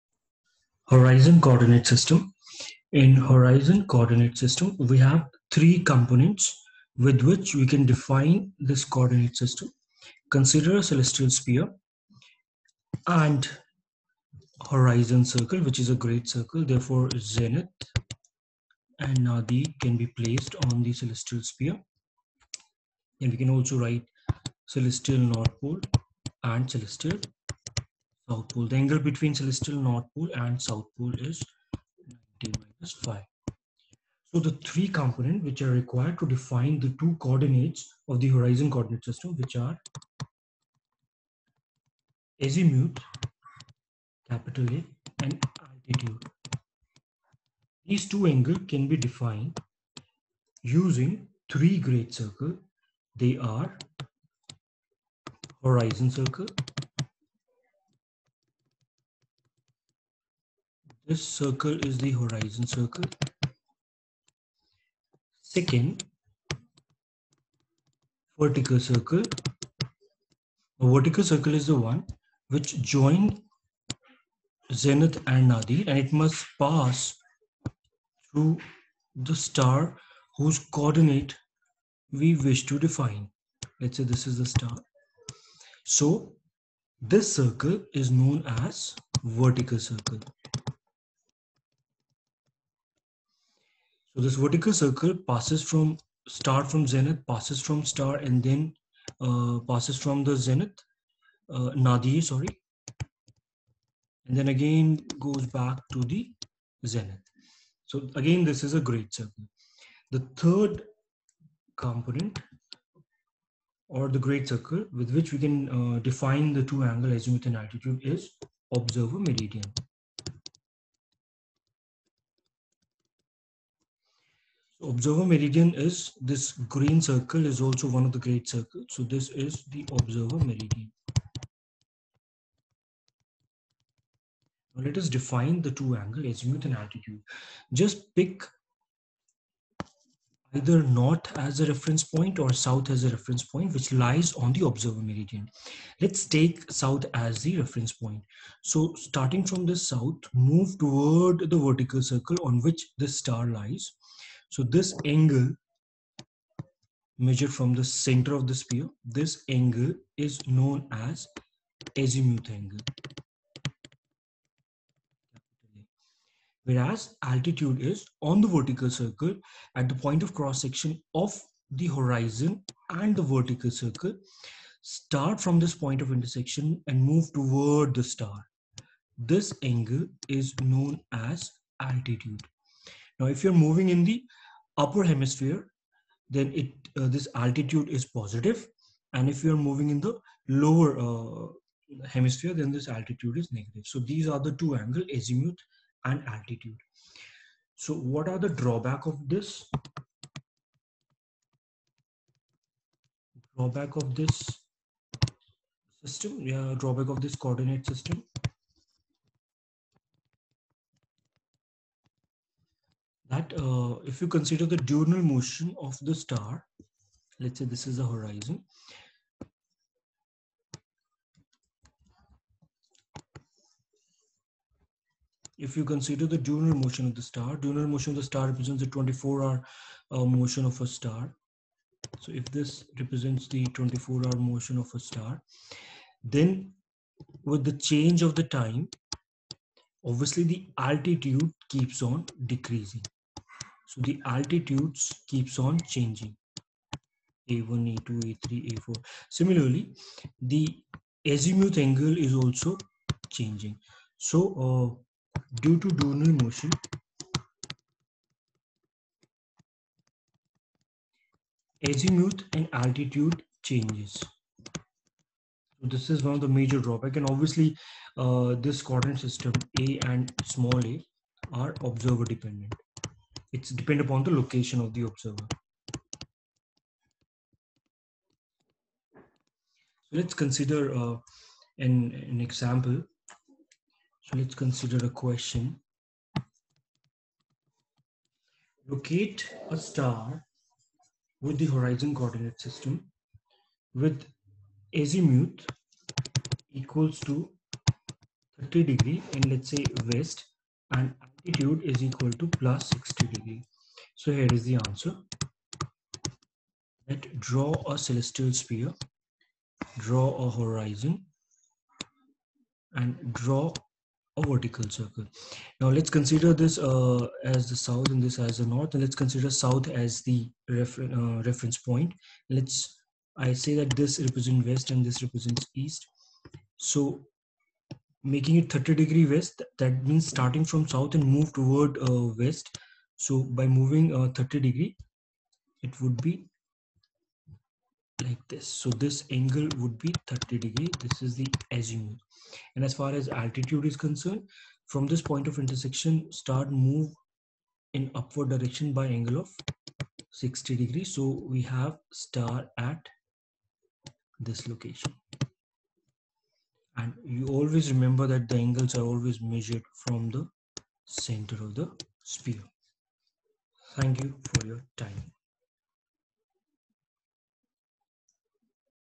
Horizon coordinate system | Astrophysics and Astronomy | Delhi University
Astronomy and astrophysics full course of undergraduate physics

Delhi university DSE semester - 5 Astrophysics course